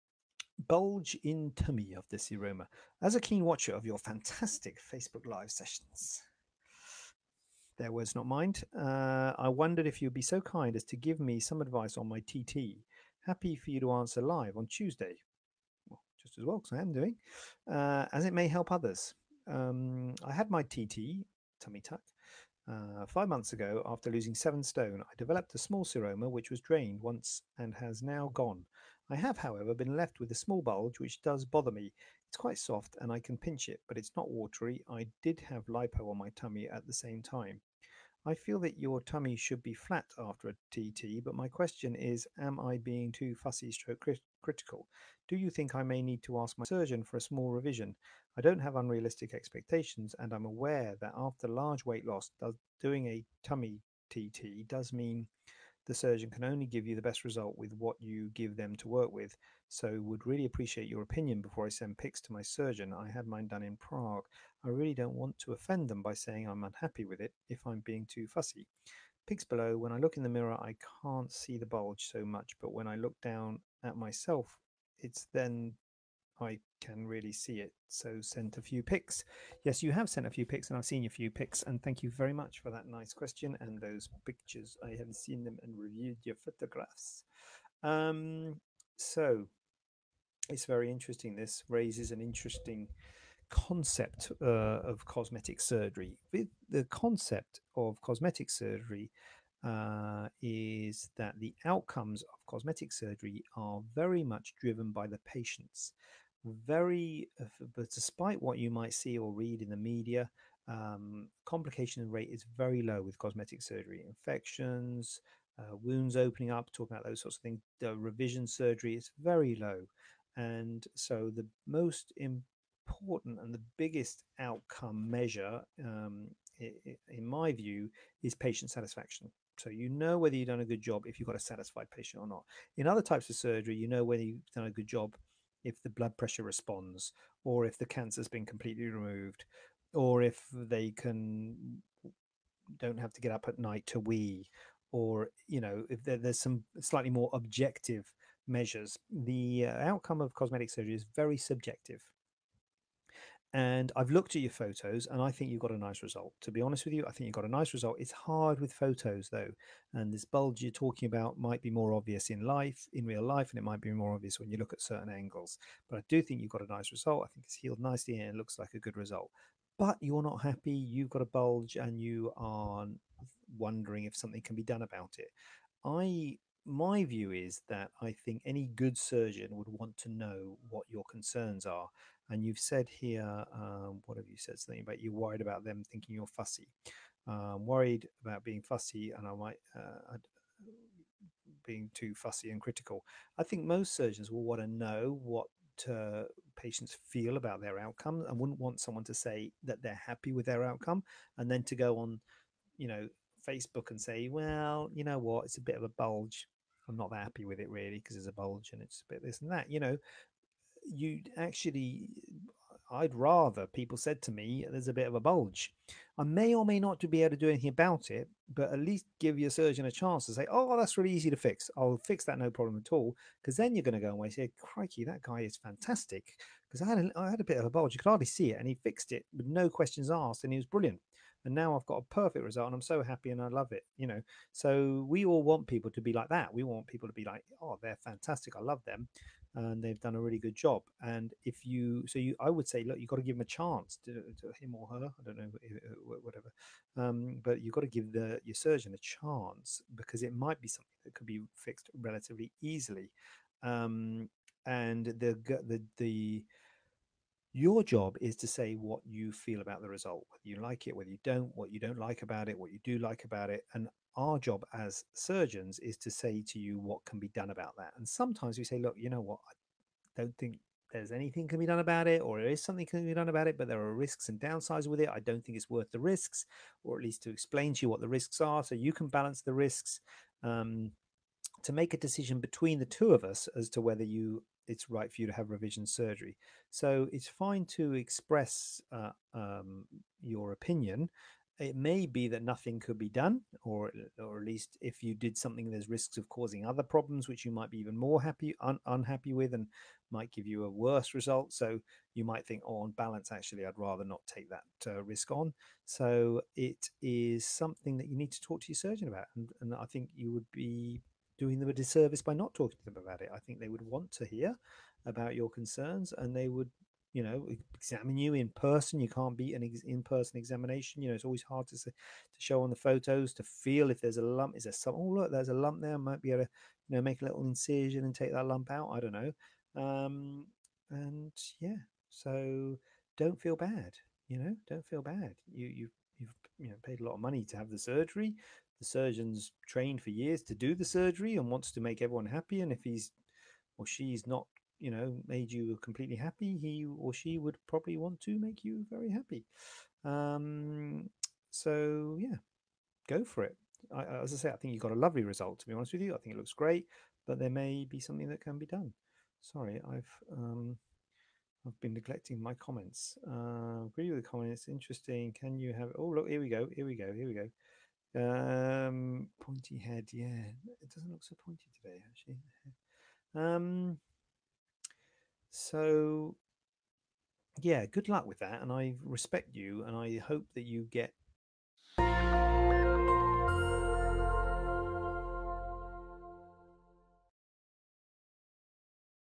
How to reduce a bulge after tummy tuck and seroma
JJ discusses the causes and how to reduce a bulge in tummy after tummy tuck and seroma

Have a question for JJ?
Watch Facebook Live every Tuesday at 7pm